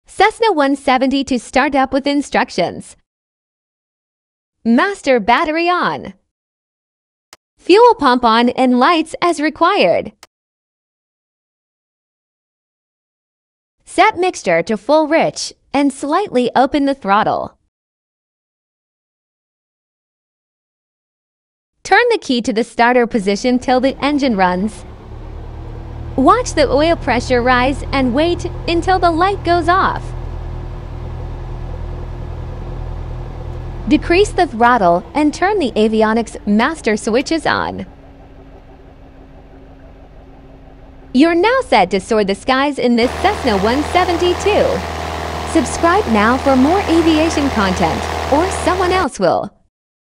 Cessna 172 startup in less than 60 seconds!
A simple instruction on how to startup your Cessna 172 in a Flightsim.